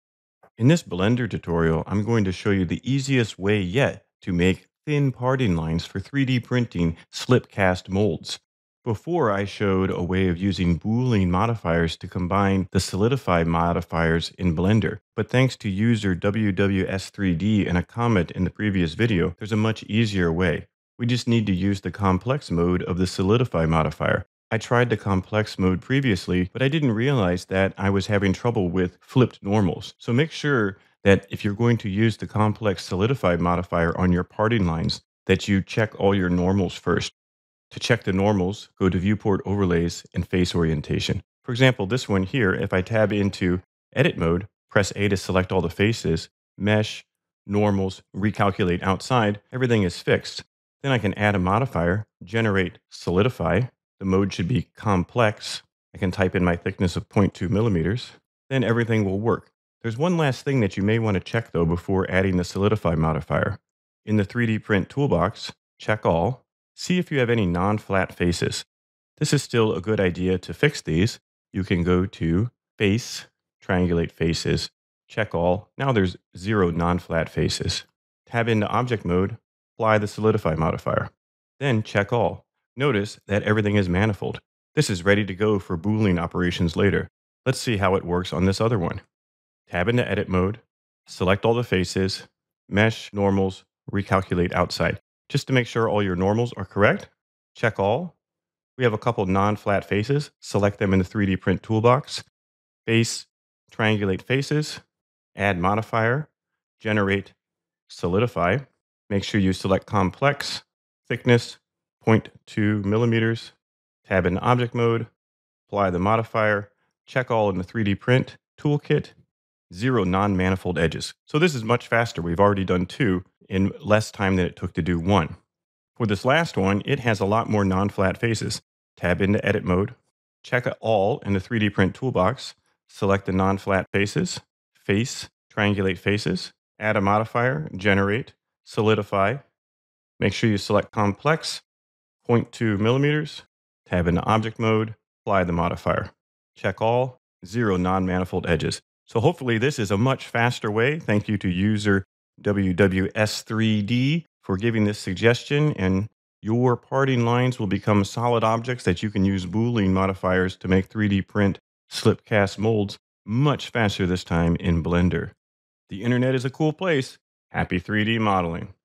Easiest Way to Make Thin Parting Lines in Blender
Thanks to @WWS3D 's comment on the previous video, there is now the fastes and easiest way to make thin parting lines for 3D printed slip cast molds in Blender. Per the comment suggestion, this tutorial shows you how to use the complex mode of the solidify modifier to create perfect parting lines every time. It is important to check for and fix flipped normals and non-flat faces, ensuring your models are ready for boolean operations.

00:00 Introduction
00:20 Using the Complex Solidify Modifier
00:37 Checking Normals
01:05 Checking for Non-Flat Faces